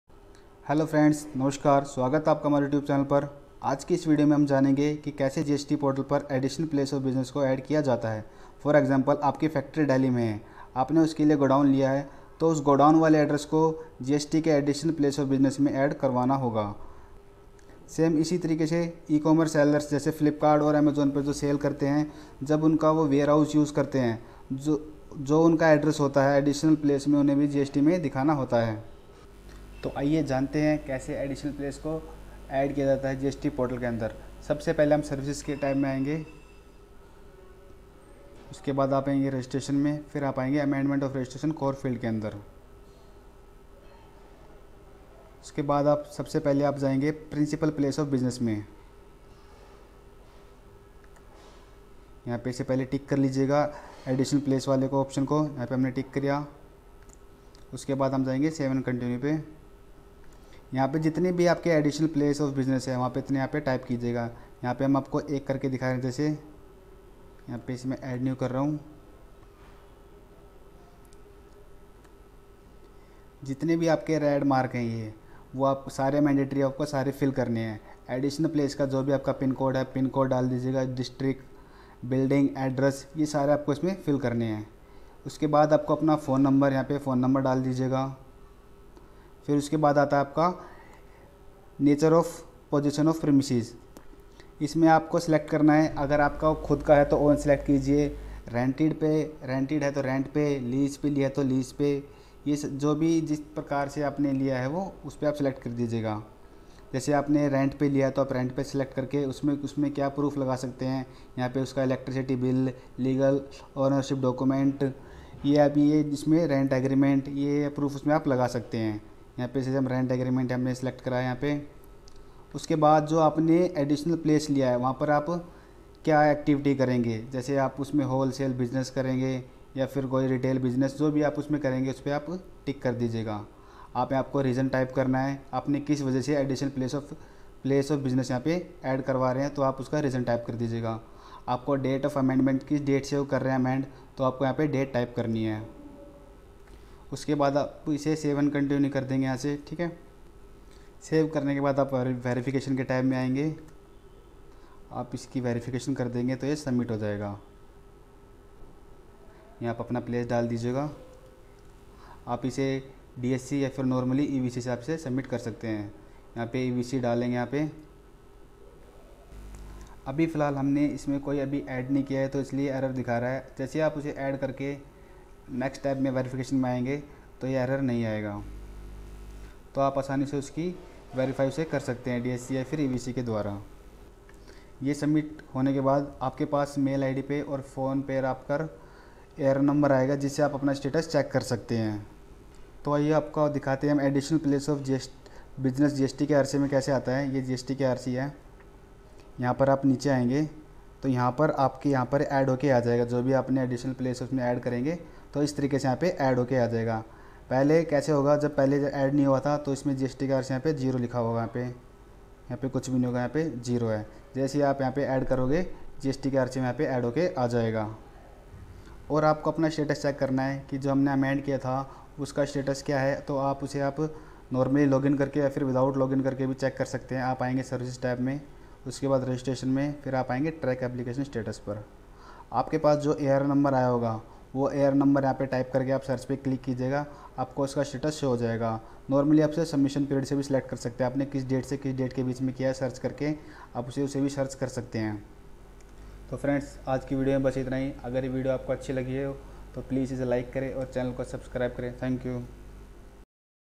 How to add additional place of business in gst/ Gst portal par ek se jyada address ko kaise add kare
In this video you know about how to add additional place of business in gst , If you got some information from this make sure to subscribe the channel and hit the like button and comment down your questions.